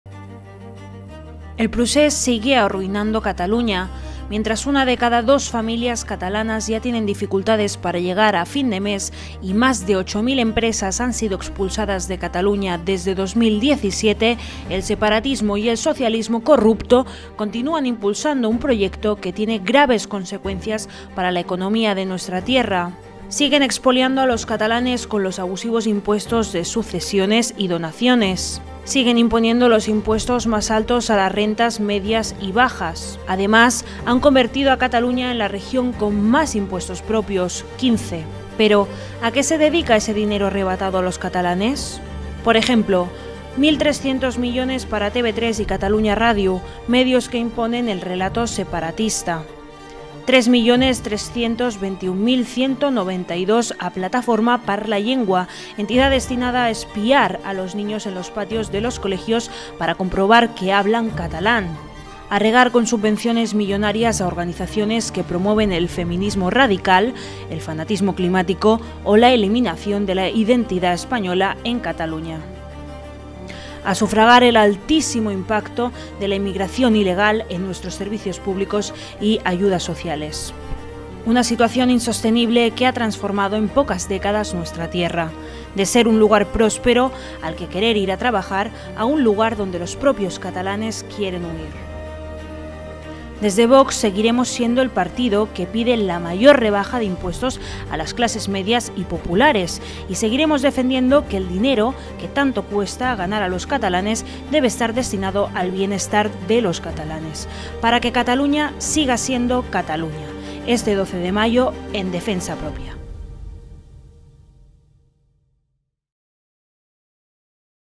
💰 Ruina económica. Han destruido Cataluña en sólo un par de décadas
Este 12M sal EnDefensaPropia.

Para que Cataluña vuelva a ser Cataluña.